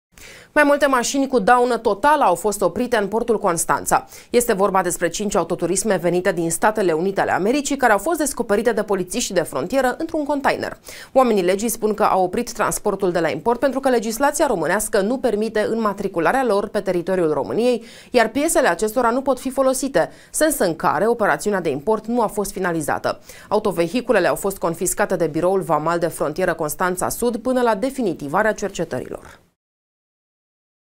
Mașini cu “daună totală”, confiscate în Port
Mai multe mașini cu daună totală au fost oprite în Portul Constanța.